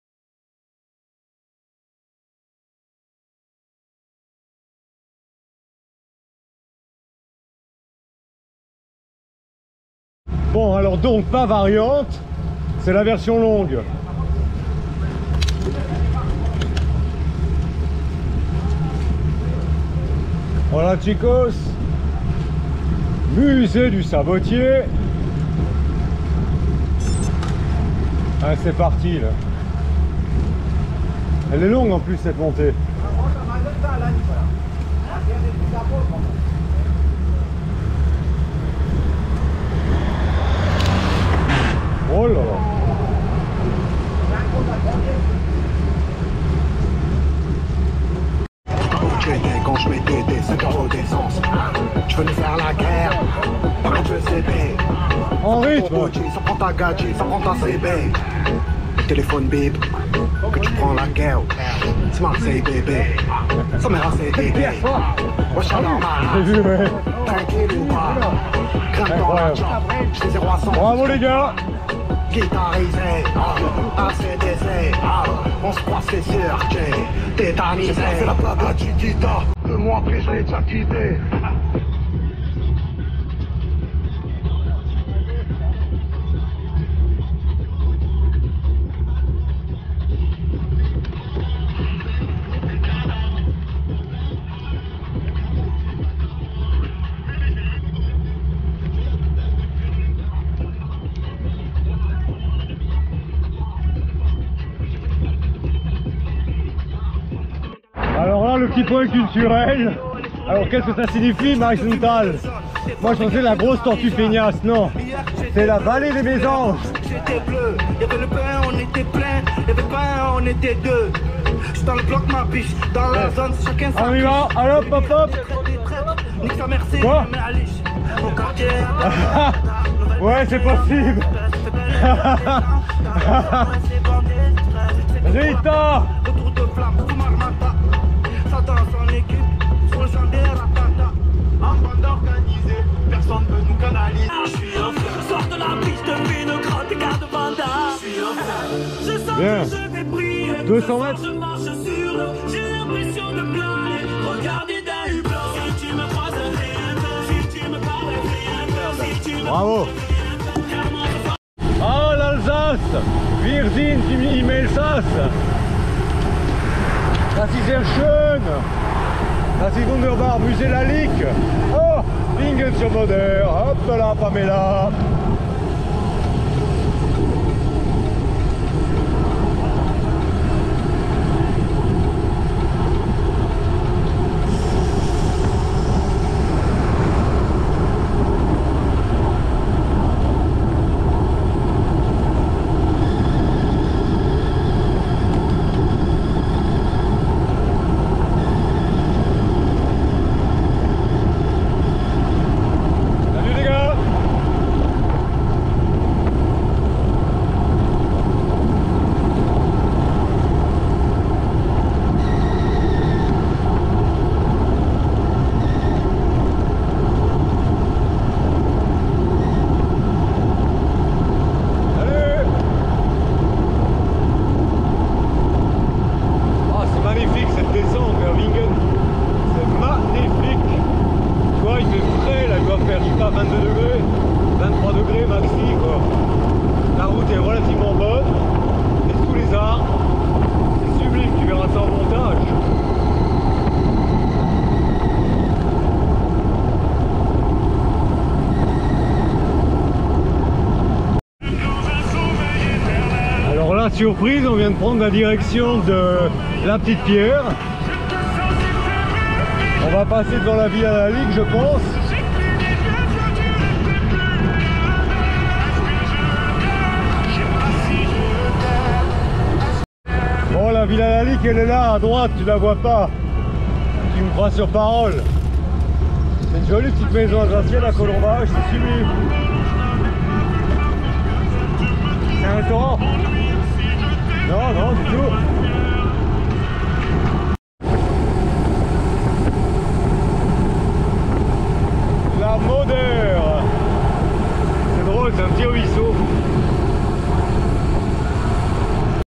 4/4 Direction Wingen/Moder puis La Petite Pierre [Full HD]
Sortie Route du 19 juin 2021 avec Michel notre Chef de File
14 cyclistes au départ ce samedi matin 8h00